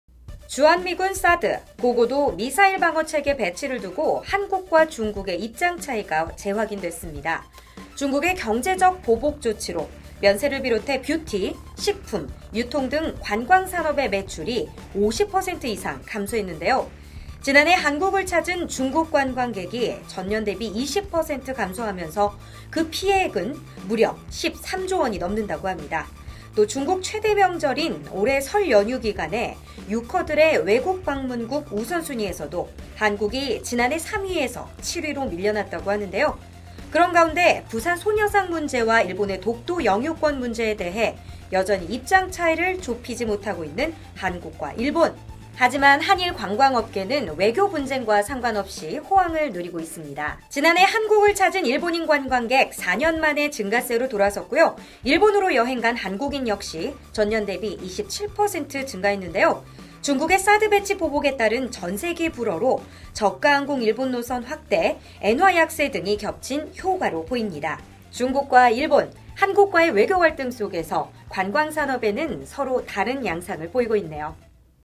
한일, 한중 외교 분쟁이 관광산업에 미치는 영향?
[한-중 외교장관 회담, 지난 2월 18일]
윤병세 외부 장관
“사드 보복 철회해라”
왕이 중국 외교부장
“사드 배치 일단 보류하라”

[중국의 경제적 보복 조치]
-한국행 전세기 운항 불허
-한국산 배터리 탑재 차량 보조금 지급 제외
-한한령(限韓令·한류콘텐츠 금지령)
-한국산 화장품 수입 불허

[중국정부 “한국행 단체관광객을 20% 축소하라”...한국, 총 13조원 외화 증발]

[한-일 외교장관 회담, 지난 2월 17일]
-부산 소녀상 문제와
-일본의 독도 영유권 문제

[한일 외교 분쟁 속, 한일 관광 증가]

[2016년 한국 방문 일본인 전년 대비 23%(229만여 명) 늘어]
[2016년 일본 방문 한국인 전년 대비 27%(509만여 명) 늘어]

[한일 관광 증가 왜?...적극적 노선 취항, 엔화 약세, 인구 고령화 등]